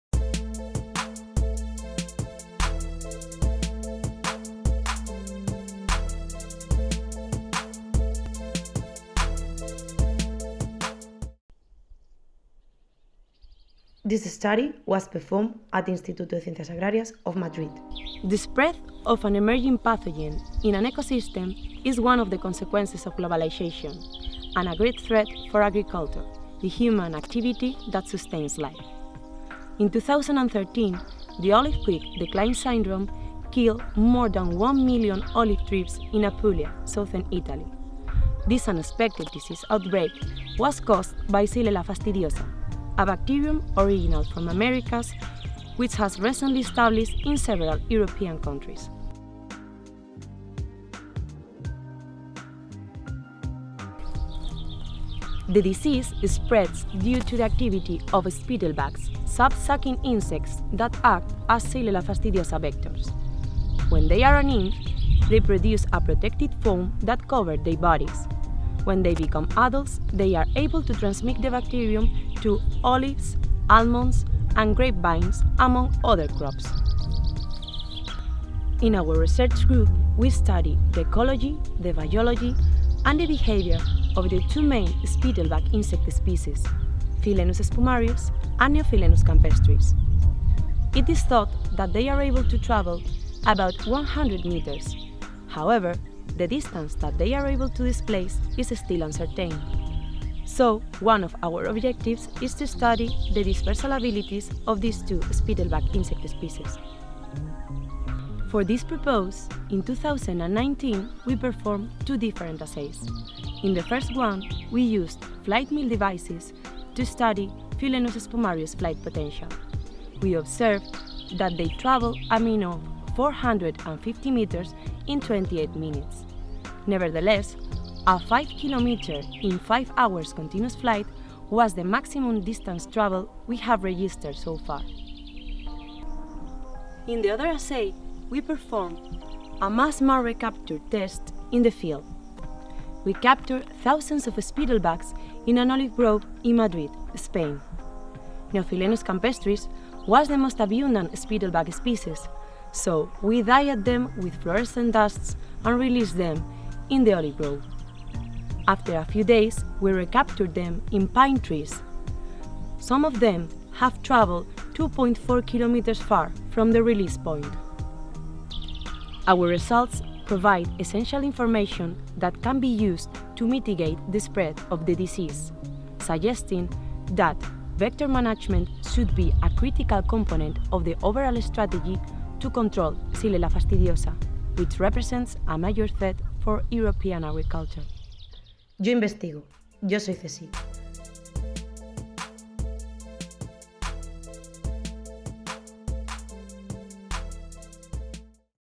III Edición Yo Investigo. Yo Soy CSIC - Clara Lago Blasco
Xylella fastidiosa is a bacterium of American origin considered as a major threat for several crops all over the world.  Its hazardous nature and public awareness can be explained because it causes serious and incurable plant diseases. Xylella is transmitted by sap sucking insects, being Philaenus spumarius the main European vector. Moreover Neophilaenus campestris is also able to transmit the bacterium. In our research group we studied the dispersal abilities of these insects, and we found out that they are able to travel 2.4km in the field, and we registered a maximum distance travelled of 5.5km under laboratory conditions. This knowledge will help to develop efficient means of control against the diseases caused by X. fastidiosa.

Studied performed at IVPP group at ICA-CSIC